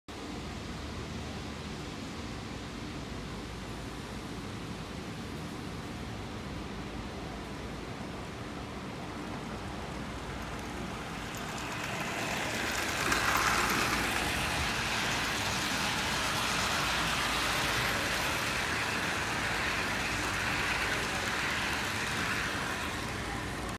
The Daylight Rides Again
After a wire broke on my Bachmann GS-4, it went out of service. Now it is fixed and will sometimes run on the Texas Transportation Museum's HO Scale Layout